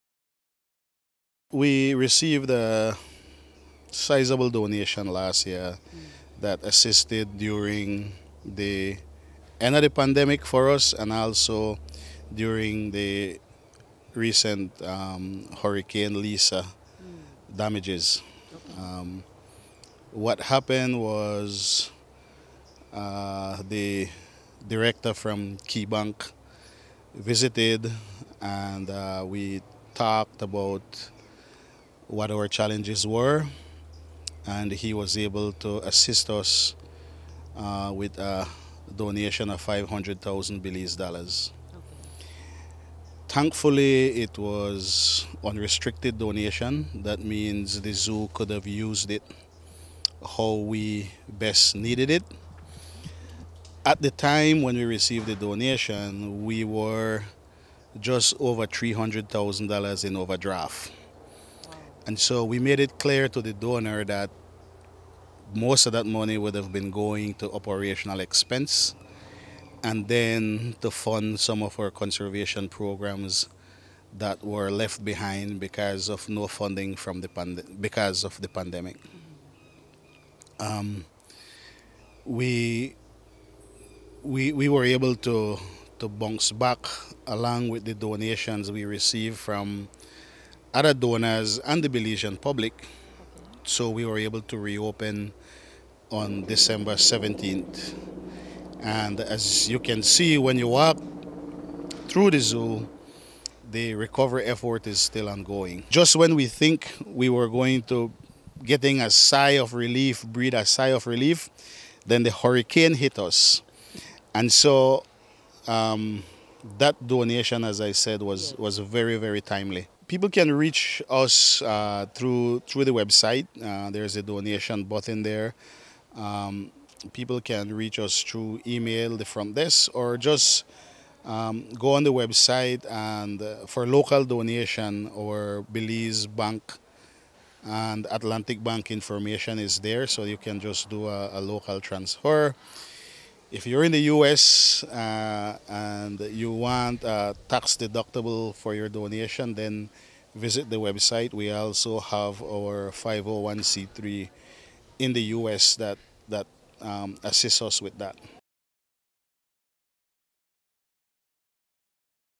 The Belize Zoo Spent A Great Part of A Donation on Cleaning Up and Rehabilitation
Poot explained that the Zoo had received a sizeable donation since the COVID pandemic, but that it was so much in the red that most of it went into balancing off its operations and then came the hurricane that required spending on cleaning up.